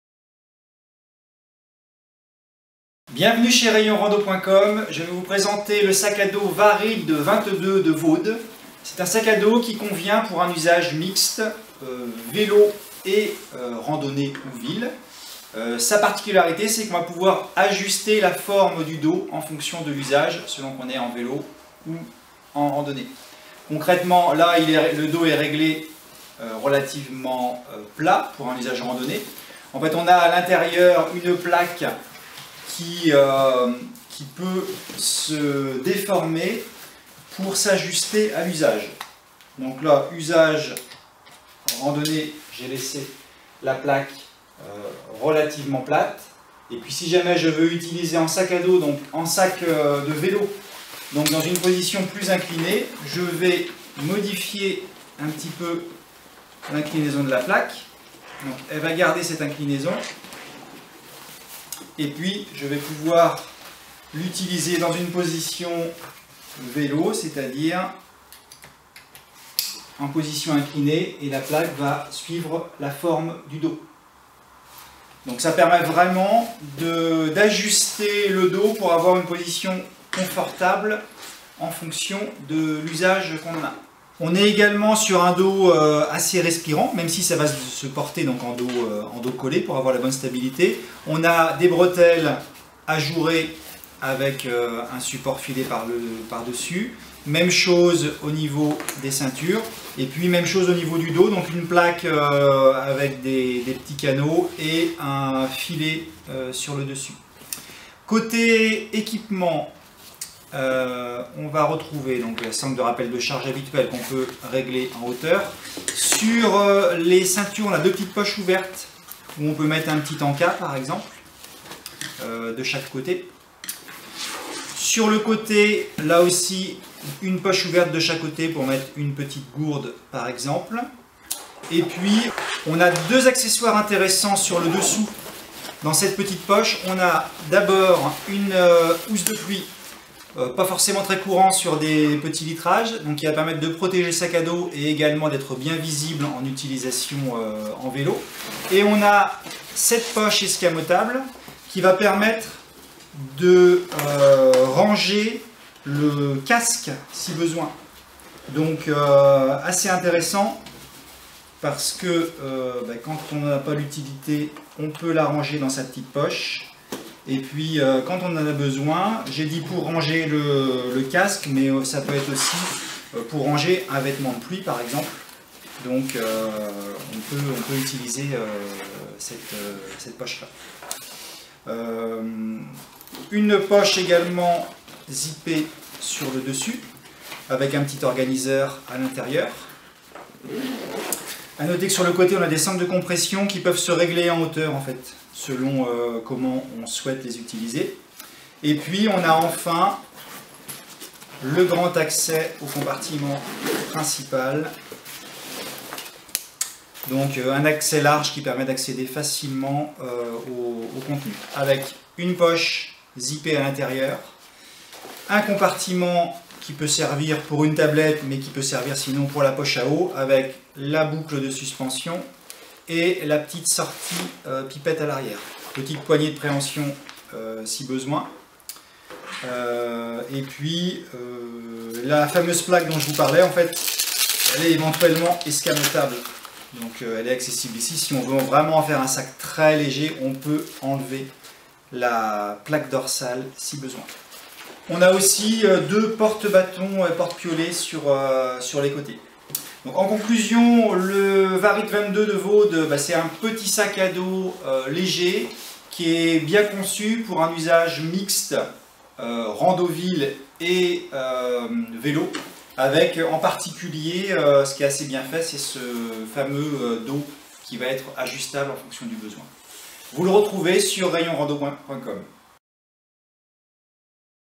RayonRando.com : Présentation du sac à dos Varyd 22 de Vaude
Sac à dos léger multi-activités ; randonnées, vélo, etc... Forme du dos inclinable en fonction de l'activité. Compartiment casque escamotable.